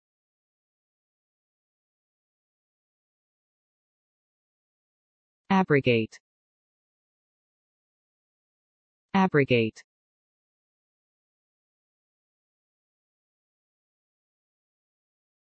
How to Read - Abrogate
Learn how to read and pronounce Abrogate in English language.

abrogate
/ˈabrəɡeɪt/
verbFORMAL
1.
repeal or do away with (a law, right, or formal agreement).
"a proposal to abrogate temporarily the right to strike"
synonyms: repudiate, revoke, repeal, rescind, overturn, overrule, override, do away with, annul, cancel, break off, invalidate, nullify, void, negate, dissolve, countermand, veto, declare null and void, discontinue; renege on, go back on, backtrack on, reverse, retract, remove, withdraw, abolish, put an end to, get rid of, suspend, end, stop, quash, scrap; disaffirm, avoid, vacate, vitiate; informalaxe, ditch, dump, chop, give something the chop, knock something on the head; rarederacinate
"the government has formally abrogated the 1977 treaty"
2.
evade (a responsibility or duty).
"we believe the board is abrogating its responsibilities to its shareholders"